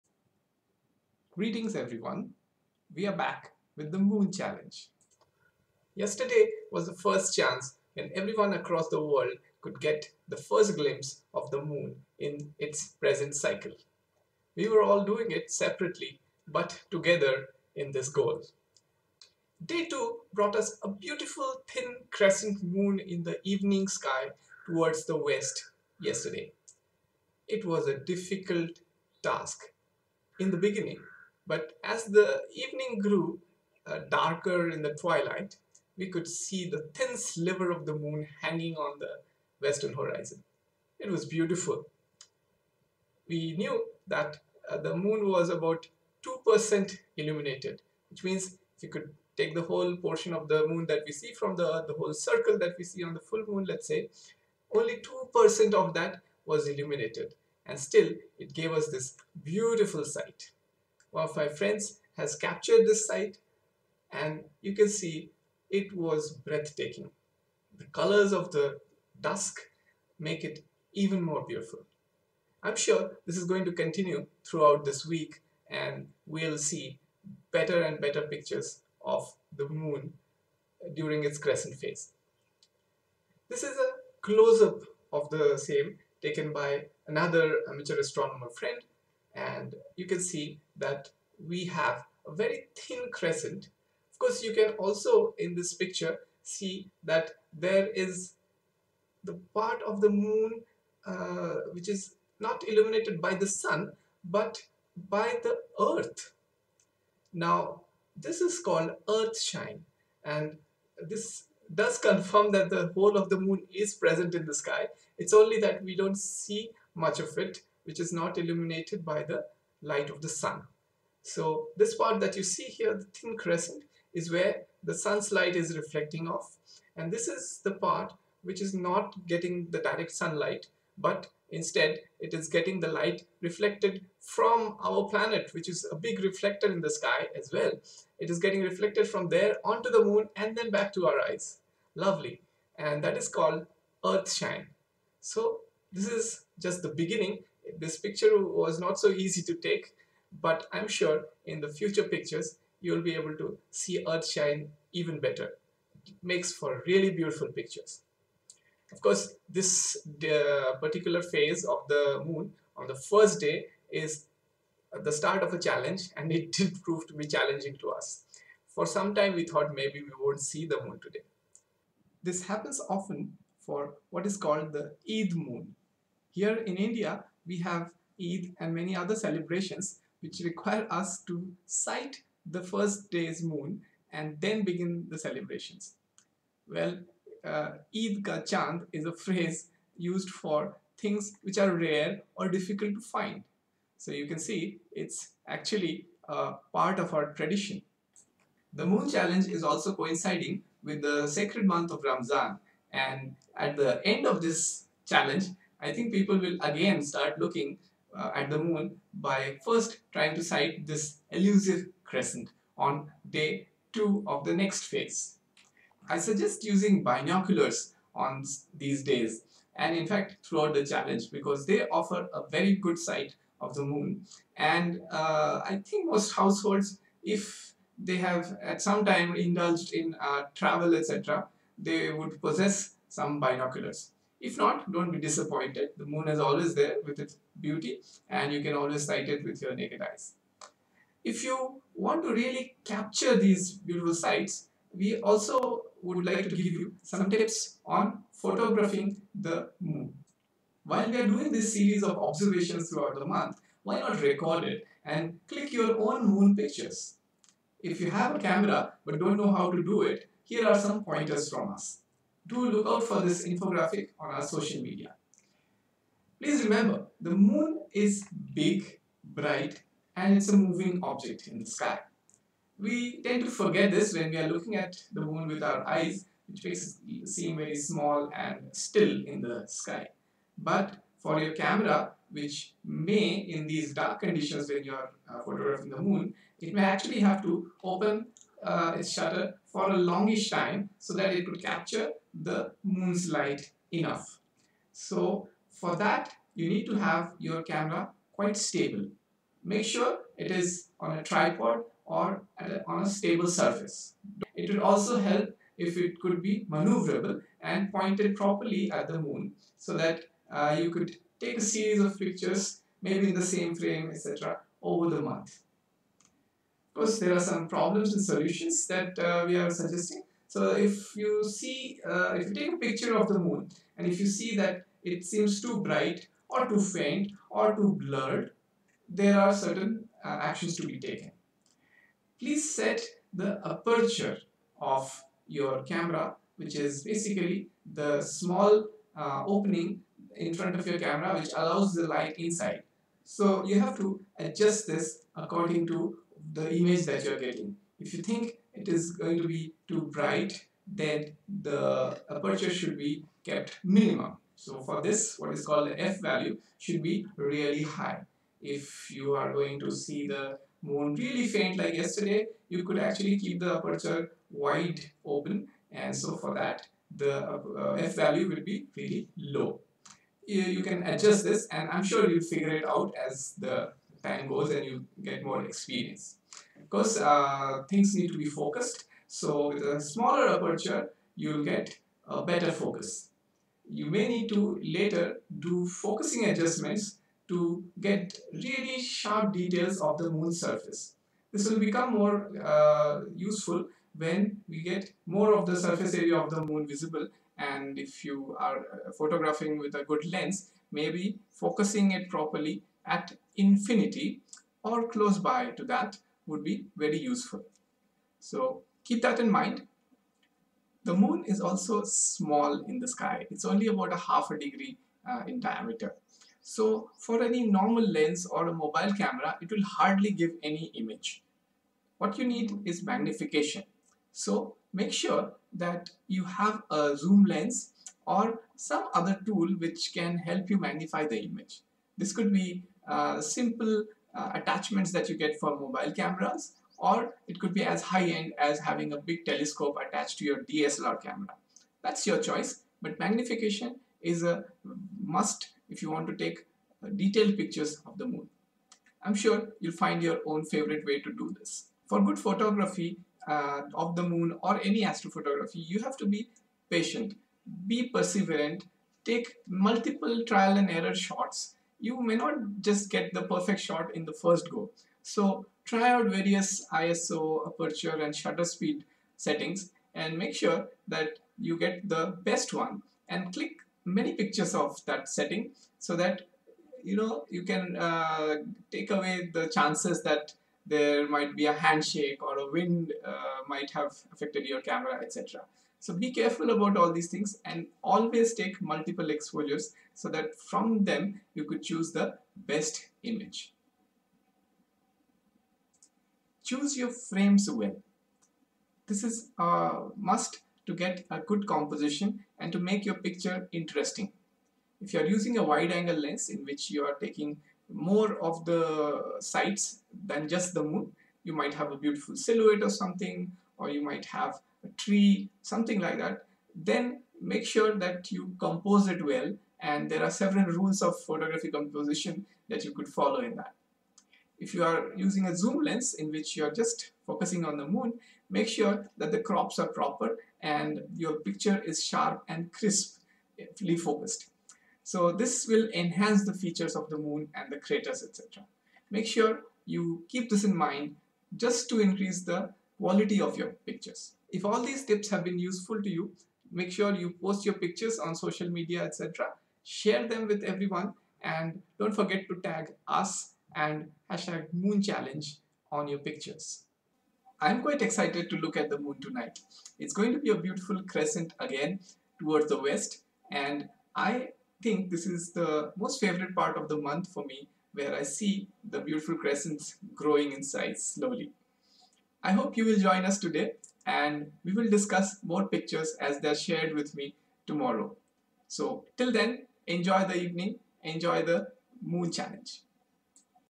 Moon Challenge - Day 02
For Day 2 we describe the first sighting of the crescent Moon after the New Moon. Some tips about Photographing the Moon are also shared.